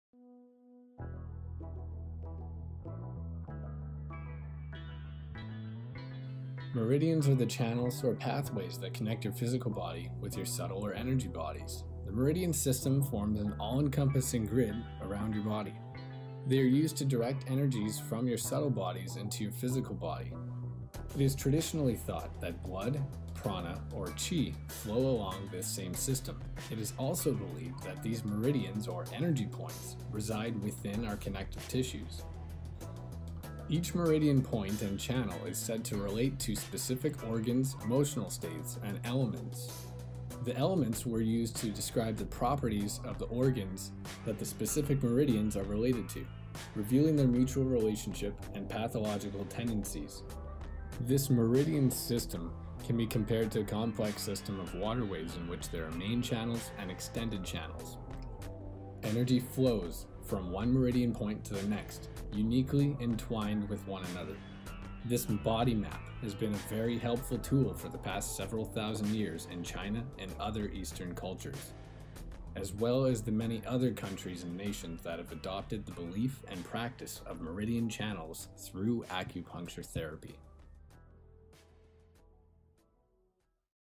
What are Meridians - Explained in 1 minute - 01
A brief introduction and explanation on "What meridians are".
Enjoy this short explanation!

We greatly appreciate any small donations made to our PayPal so that our work continues.

This is the first video of our series called "Explained in a minute".